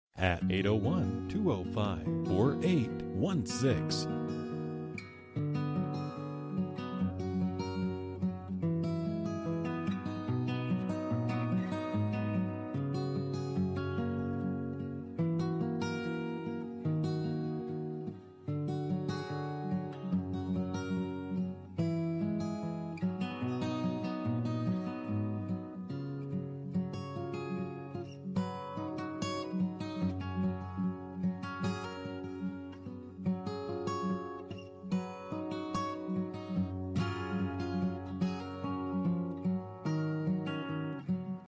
Residential for sale - 4061 E BARTON CREEK, Eagle Mountain, UT 84005
Quiet cul de sac in newer but established Highland on the Green neighborhood adjacent to Golf  Course.  HOA includes swimming pool, picnic area, basketball. Great building lot.

Bedrooms: 0
Bathrooms: 0.00
Square Feet: 0
Price: $120,000